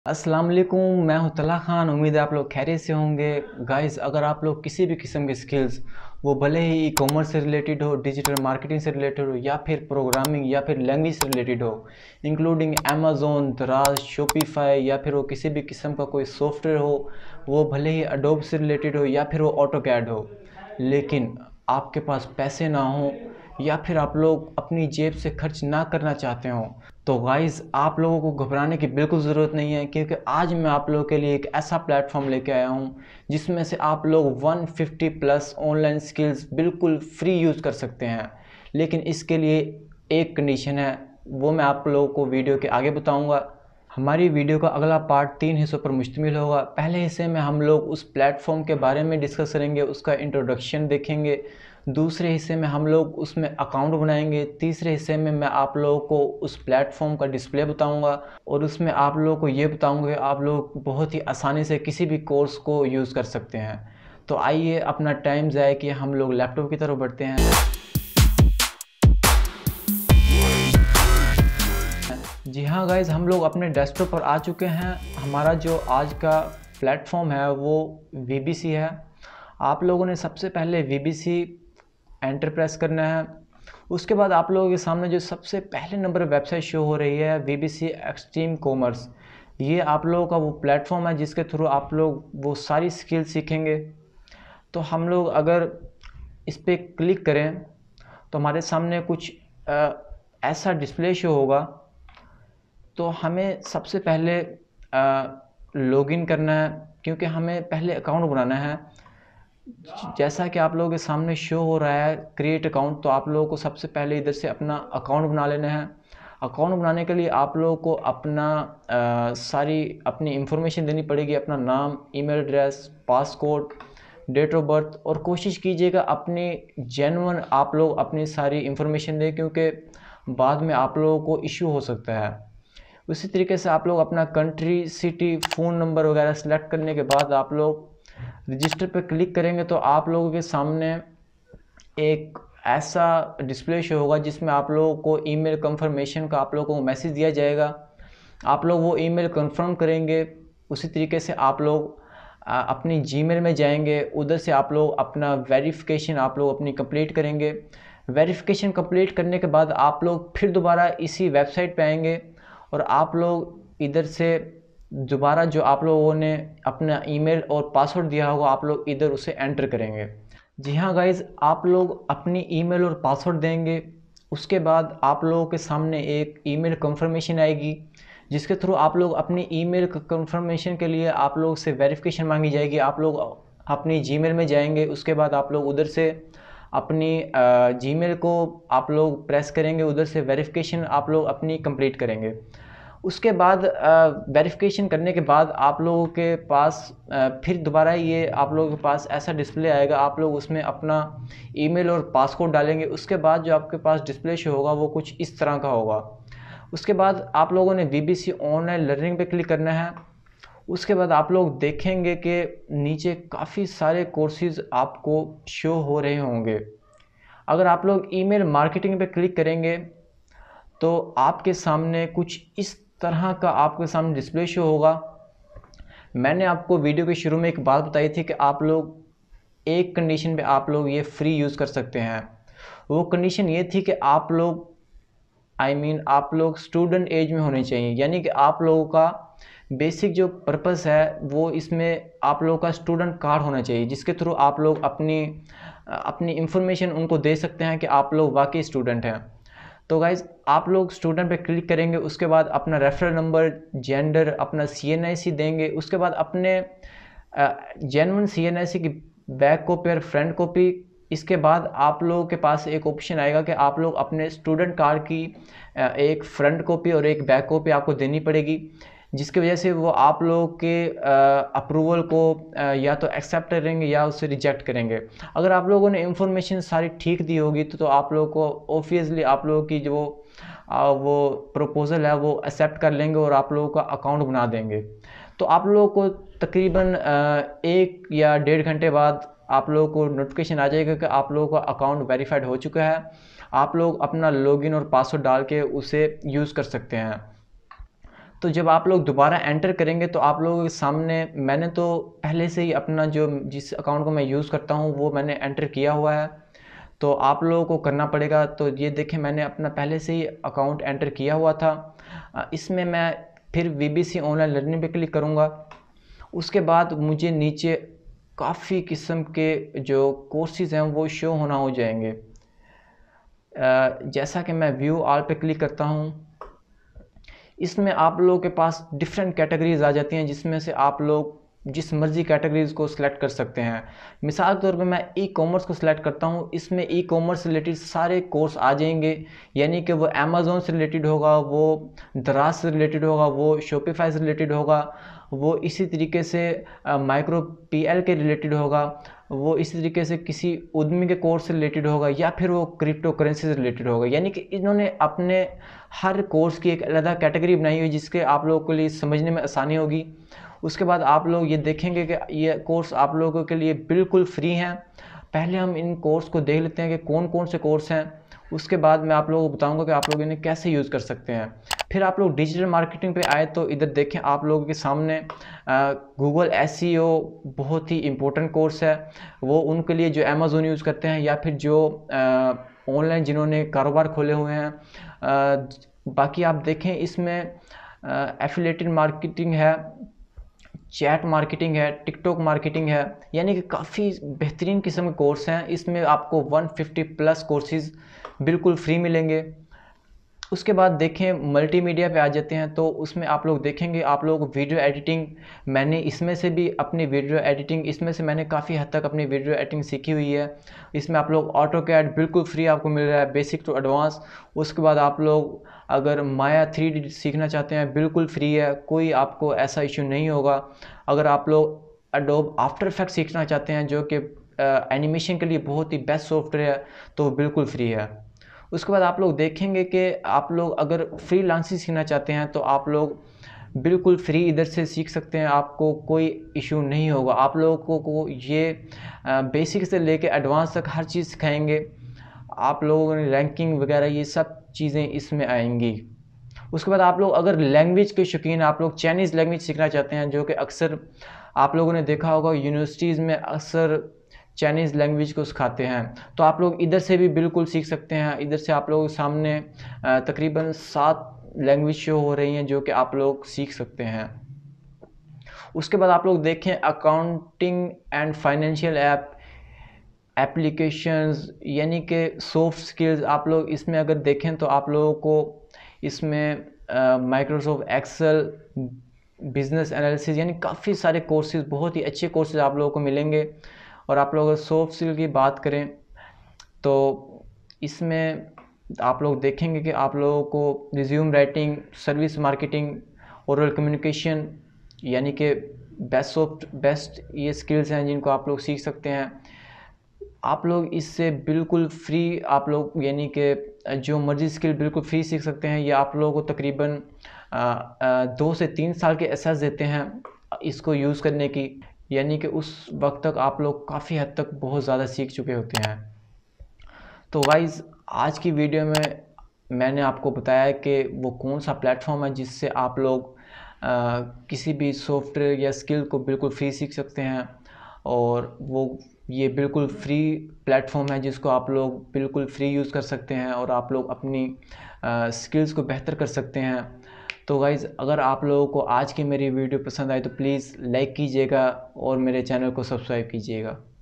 Best FR** WEBSITE to Learn a New Skills | Learn any Top Skills for Fr** | Talha
In this video, I shall tell you  about best website to learn 150+ new free. Through this  website , you will be able to learn almost  every top skills. In this video , I shall tell you how to learn any top skills for free and best free courses.